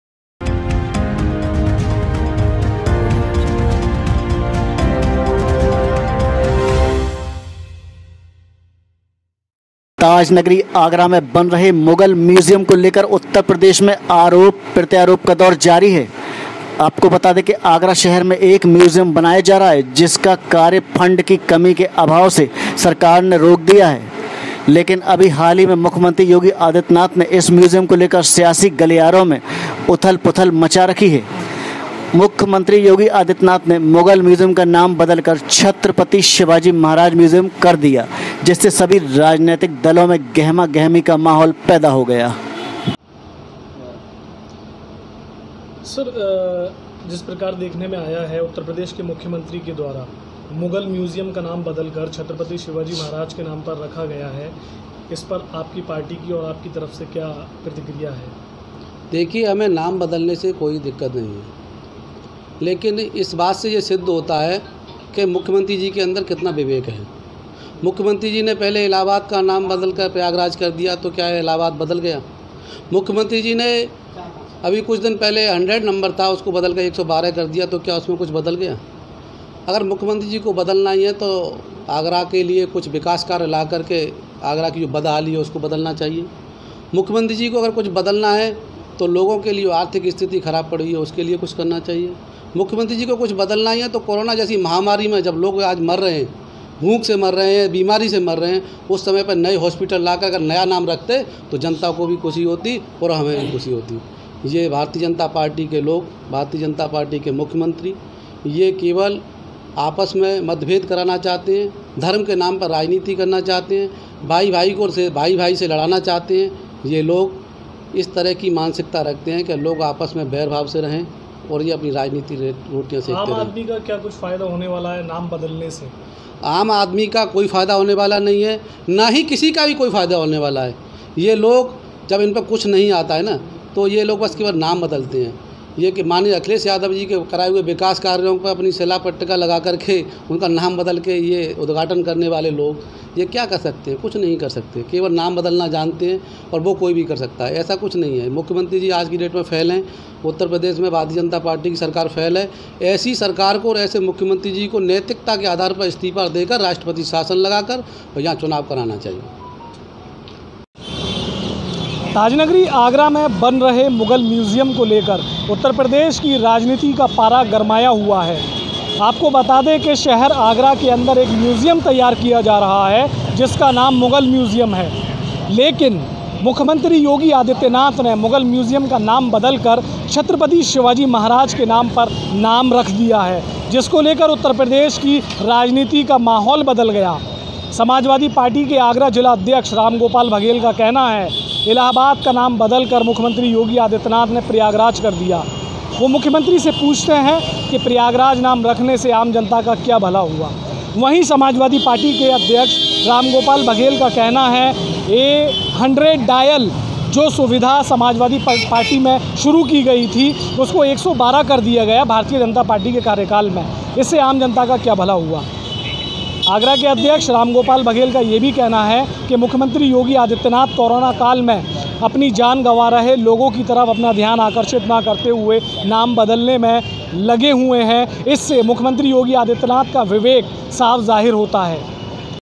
आगरा : बन रहे मुग़ल मियूजियम लेकर लेकर उत्तर प्रदेश में आरोप प्रत्यारोप का दौर जारी है !!
आगरा में बन रहे मुग़ल मियूजियम लेकर लेकर उत्तर प्रदेश में आरोप प्रत्यारोप का दौर जारी है !!

आपको बता दें कि आगरा शहर में एक म्यूजियम बनाया जा रहा जिसका कार्य फंड की कमी के अभाव से सरकार ने रोक दिया था

लेकिन अभी हाल ही में मुख्यमंत्री योगी आदित्यनाथ ने इस म्यूजियम को लेकर सियासी गलिहारों में उथल पुथल मचा दी है